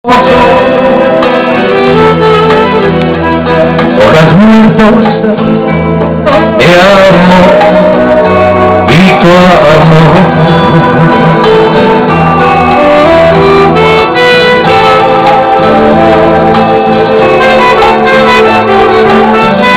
São Lourenço, MG, Parque das Águas.
Eu filmando "show" do cantor Nino, estando a minha Mãe sentada à mesa em frente, no Restaurante "Fubas", situado próximo ao Parque das Águas da Cidade de São Lourenço, MG, Brasil; durante o Carnaval de 2009.